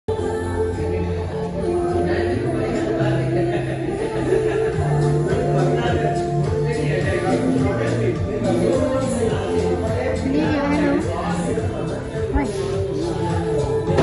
Singing a song
hello guys 🤗
m singing a song in restaurant
my mom captured this moment 😂😂😂
capture| moments |singing |boy song |fun |yourself |love care mother |
m Inspired by these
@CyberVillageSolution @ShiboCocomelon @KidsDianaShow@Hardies